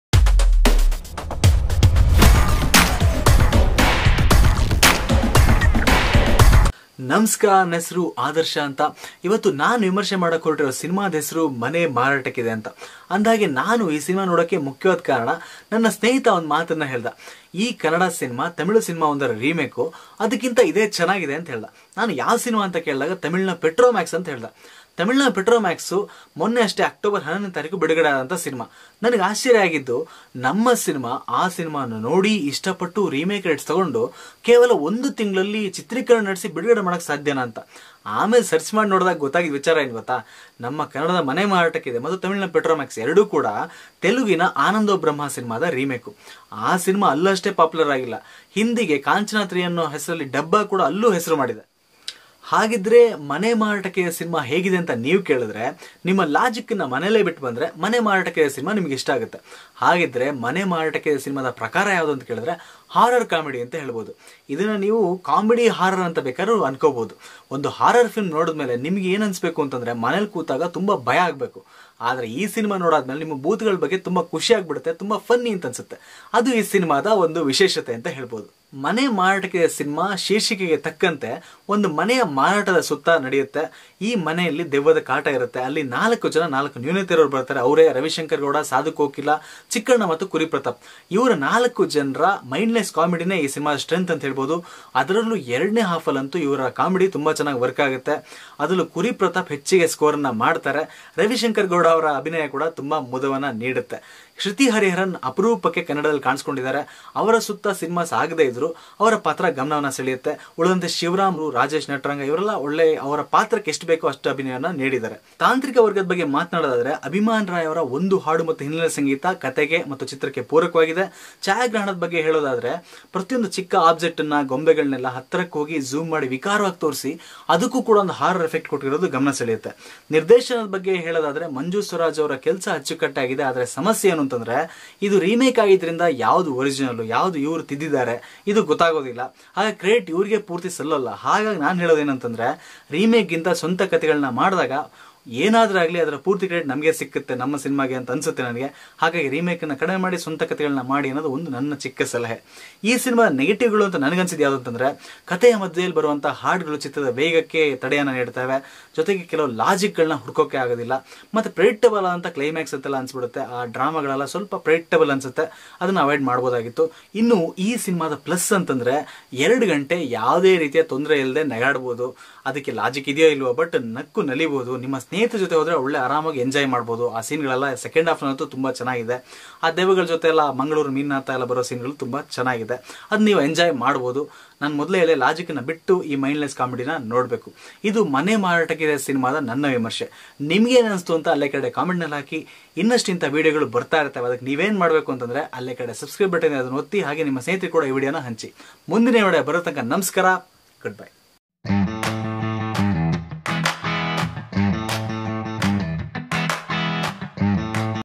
Mane Maratakkide Movie Review | Chikkanna | Sadhu Kokila | Kuri Prathap | Shruthi hariharan |Adarsha
Here is a review of the movie 'Mane Maratakkide' starring Kuri Prathap,Chikkanna,Sadhu Kokila,Ravishankar Gowda,Shivaram,Shruthi hariharan.If U end up liking this video do subscribe to my channel.Let me know Ur opjnion in comments section.

ಮನೆ ಮಾರಾಟಕ್ಕಿದೆ ಸಿನಿಮಾದ ವಿಮರ್ಶೆ ವಿಡಿಯೋ ಇಲ್ಲಿದೆ.ನಿಮಗಿದು ಇಷ್ಟವಾದಲ್ಲಿ ಇನ್ನಷ್ಟು ಇಂತಹ ವಿಡಿಯೋಗಳಿಗಾಗಿ ನನ್ನ ಚಾನೆಲ್ ಗೆ subsribe ಆಗಿ.

ಸಿರಿಗನ್ನಡಂ ಗೆಲ್ಗೆ ಸಿರಿಗನ್ನಡಂ ಬಾಳ್ಗೆ.

ಆದರ್ಶ ಯು ಎಂ
ಜಯಂತ್ ಕುಮಾರ್ ಟಿ.ವಿ